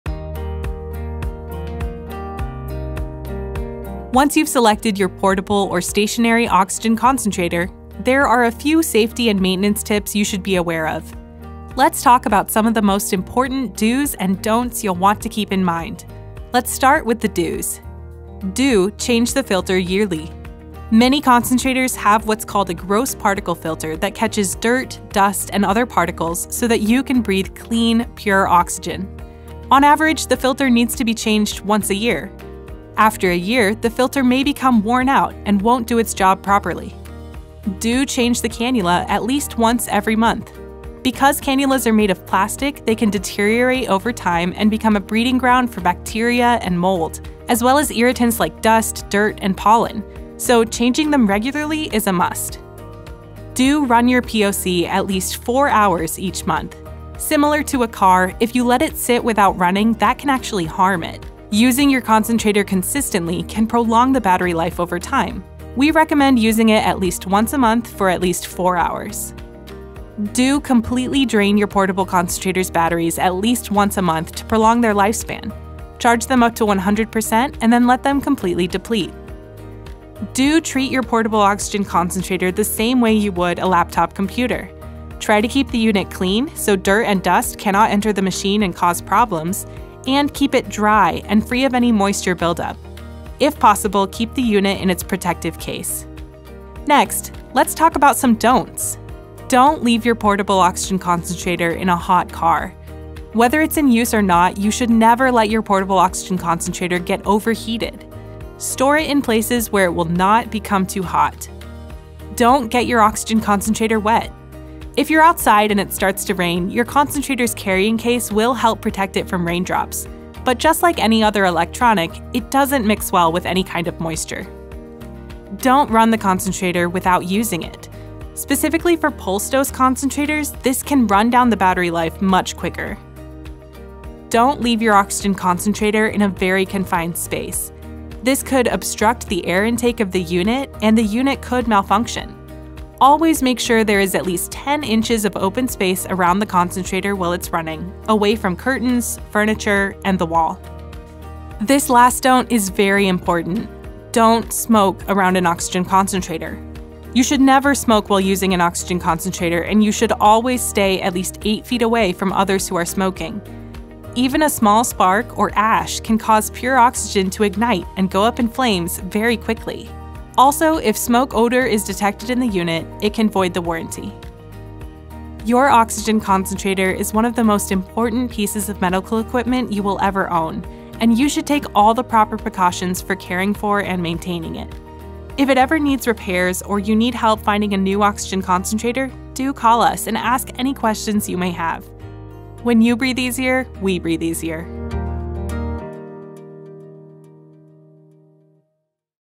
The DOs and DON’Ts of Oxygen Concentrators
Once you’ve selected your portable or stationary oxygen concentrator, there are a few safety and maintenance tips you should be aware of. Let’s talk about some of the most important do’s and don’ts you’ll want to keep in mind.

0:18 Do change the filter yearly
0:38 Do change the cannula at least once every month
0:54 Do use your POC at least 4 hours each month
1:09 Do completely drain your batteries at least once a month
1:21 Do treat your portable oxygen concentrator the same way you would a laptop computer
1:31 Don't leave your portable oxygen concentrator in a hot car
1:52 Don't get your oxygen concentrator wet.
2:05 Don't run the concentrator without using it
2:14 Don't keep your oxygen concentrator in a small space
2:31 Don't  smoke around an oxygen concentrator